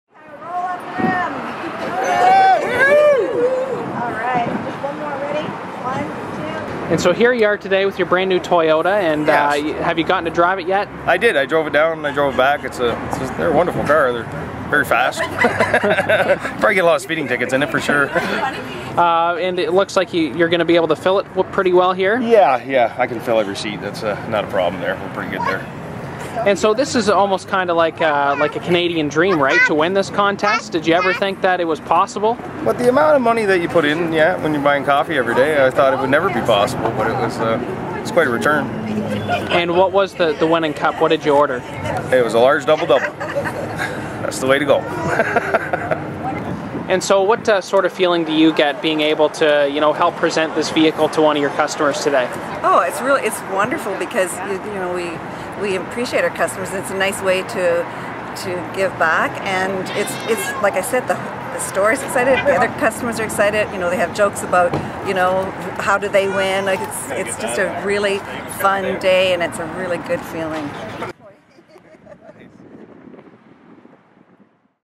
Roll Up the Rim Winner
Brennan Brown of Woodstock rolls away in a new Toyota Matrix.